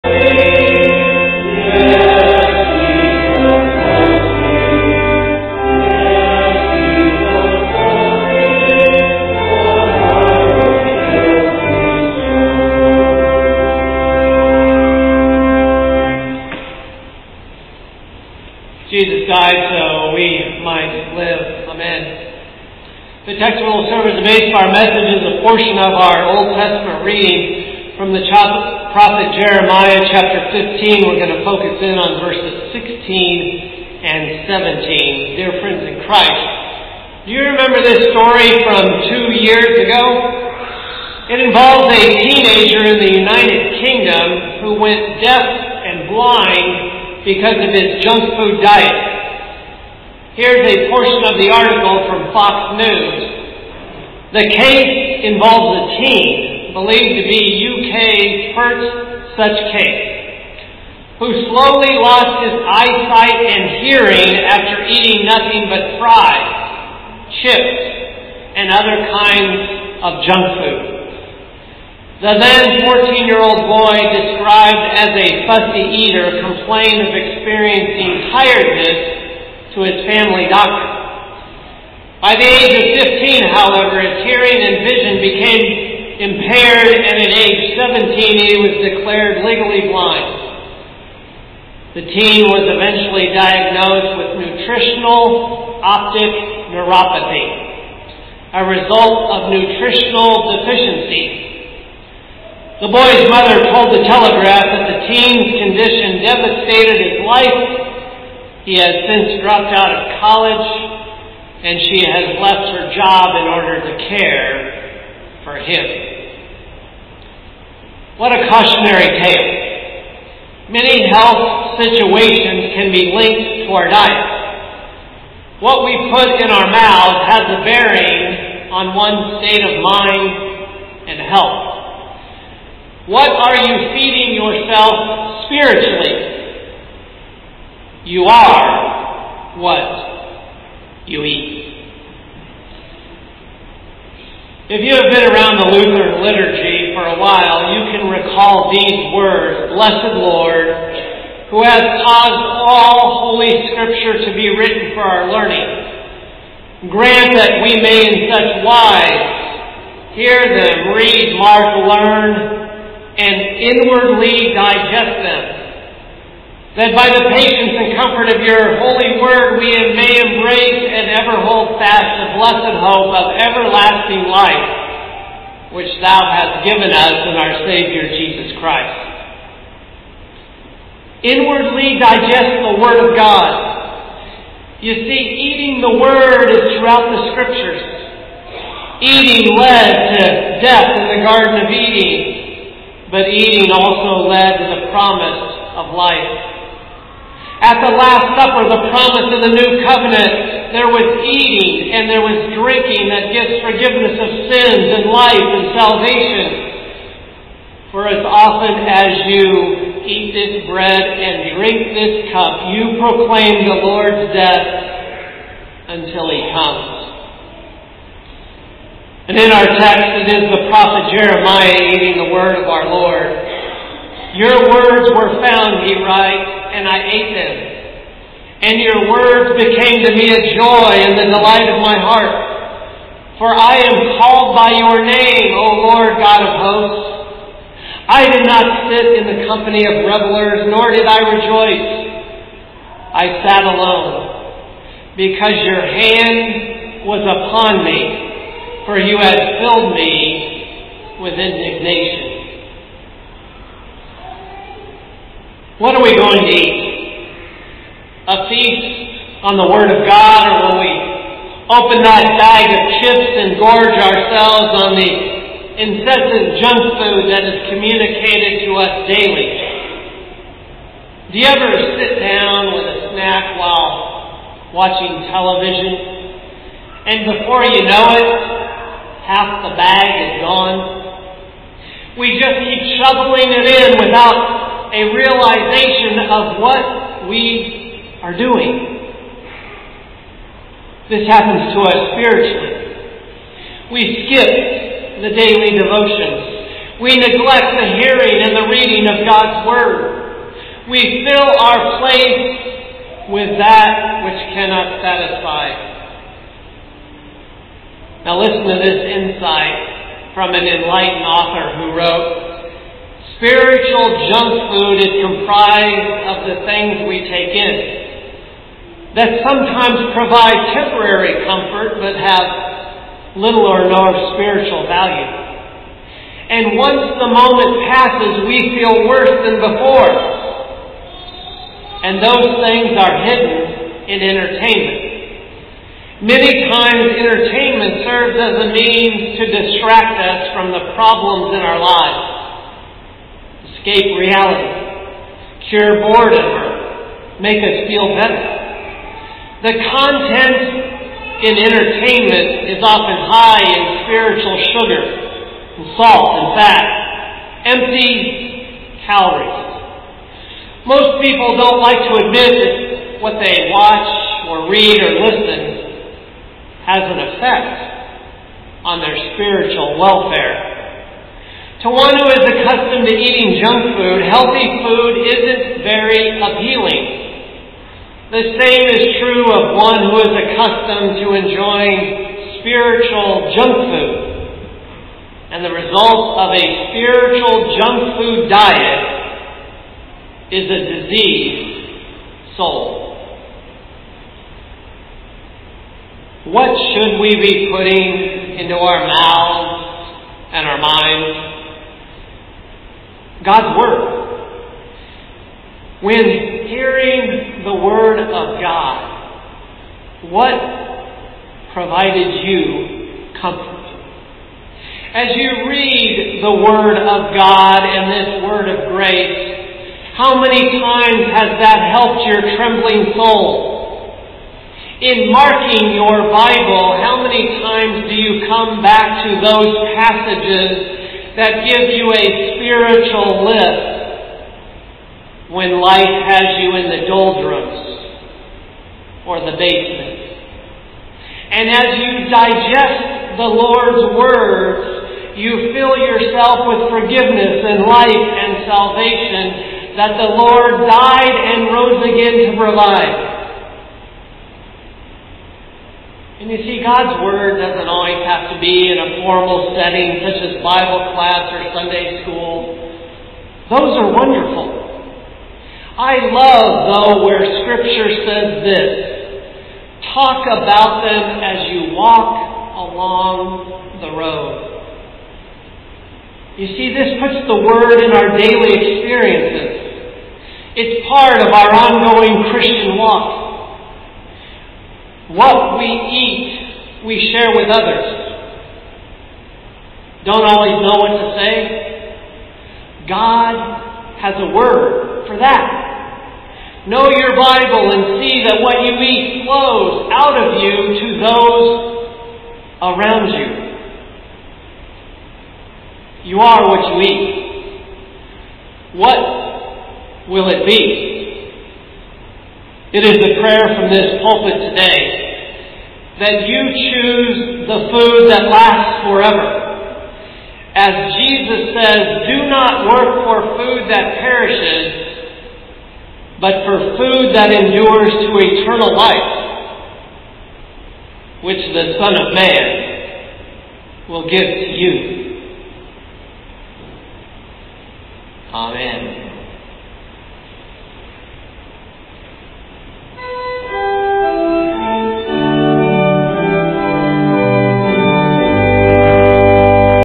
Sermon 8.30.2020 -- You Are What You Eat
Sermon video for the 13th Sunday after Pentecost